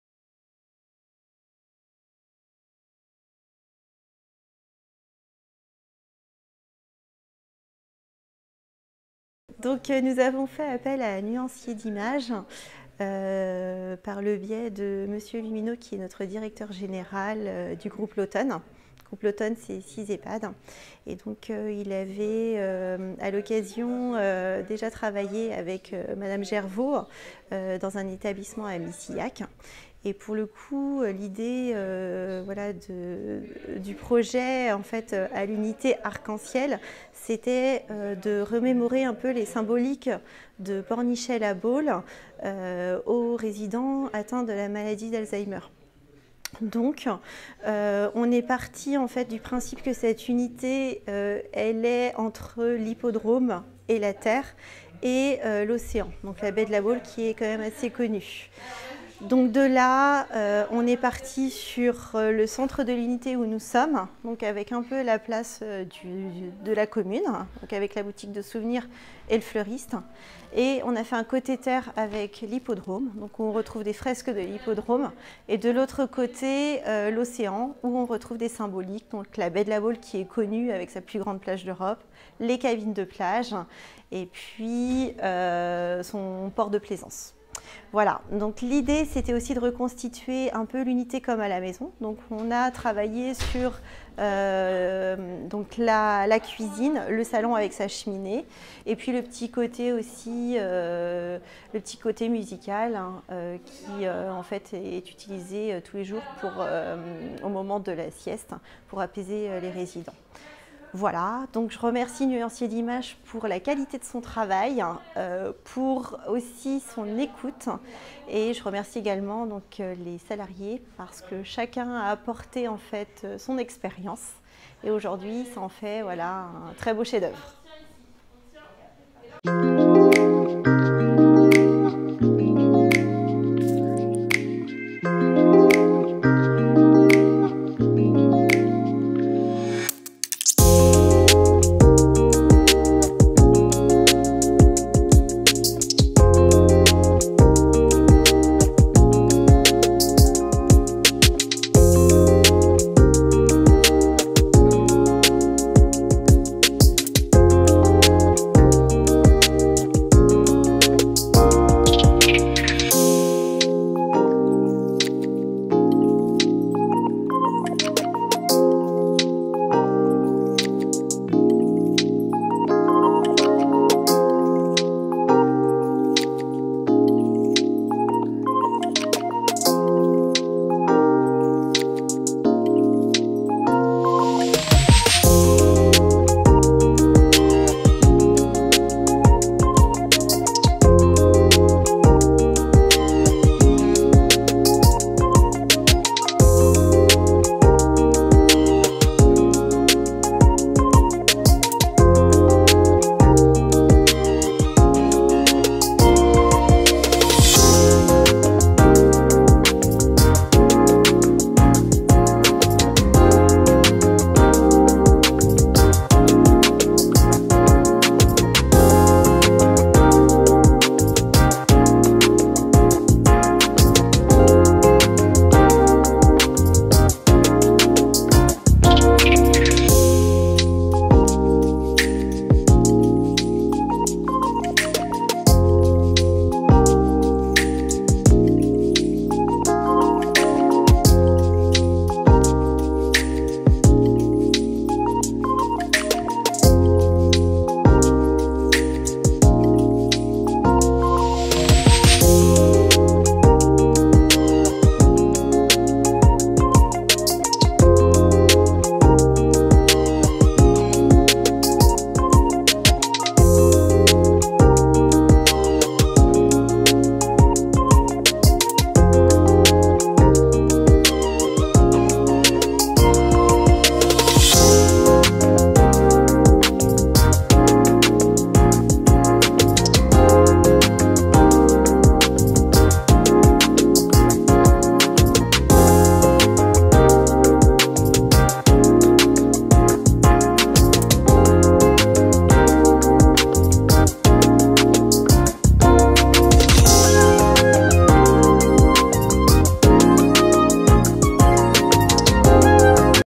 Résidence de la côte d'Amour (Pornichet 44)
Découvrez la décoration murale sur mesure par Nuancier d'Images pour cet établissement au cœur de Pornichet. Trompe l’œil en adhésif ou papier peint, lieux emblématiques de Pornichet dessinés par nos artistes et produit dans notre atelier. Retrouvez l'interview de la directrice !